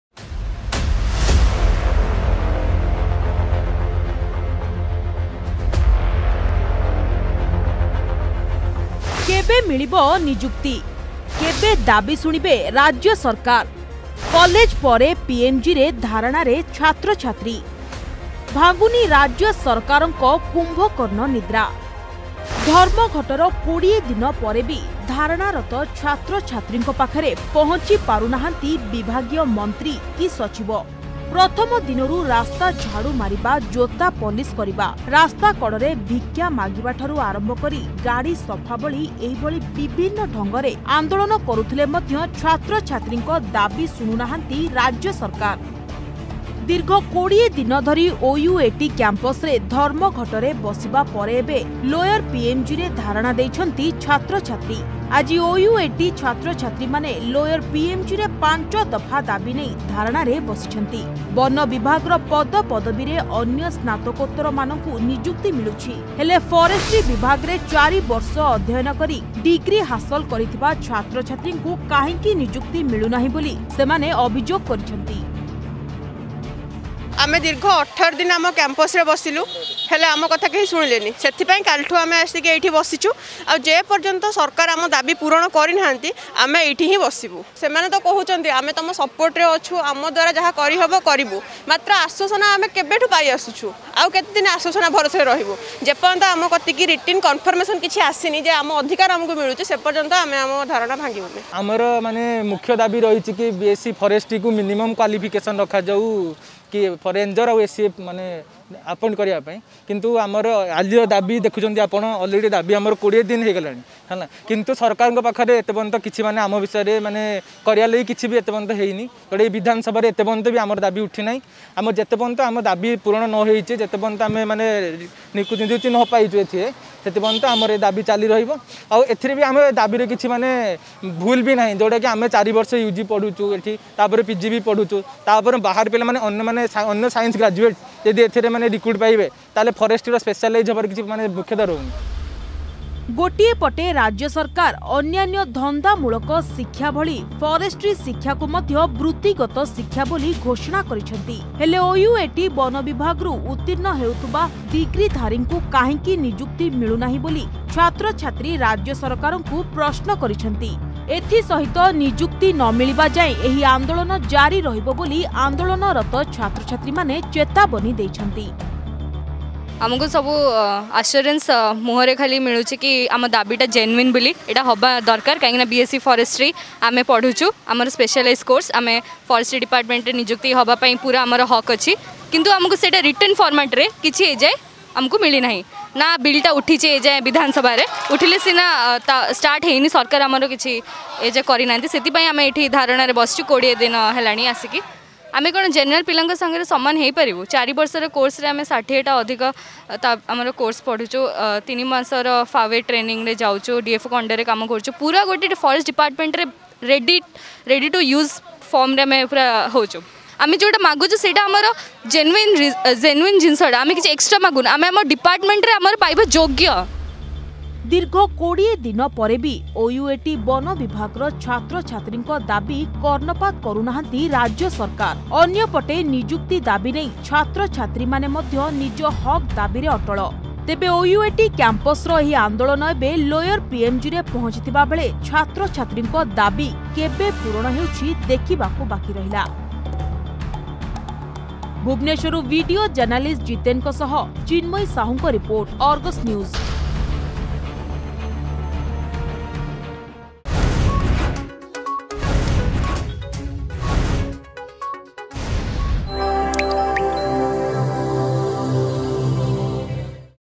କେବେ ମିଳିବ ନିଯୁକ୍ତି
ଲୋୟର ପିଏମଜିରେ ବନବିଭାଗ ଛାତ୍ରଛାତ୍ରୀ...ଦୀର୍ଘ 18 ଦିନ କ୍ୟାମ୍ପସ ପରିସରରେ ବସିବା ପରେ ଏବେ ଲୋୟର ପିଏମଜିରେ ବନବିଭାଗ ଛାତ୍ରଛାତ୍ରୀ ଧାରଣା ଦେଇଛନ୍ତି । 5 ଦଫା ଦାବି ପୂରଣ ନହେବା ଯାଏଁ ଆନ୍ଦୋଳନ ଜାରି ରଖିବାକୁ ଦେଇଛନ୍ତି ଚେତାବନୀ...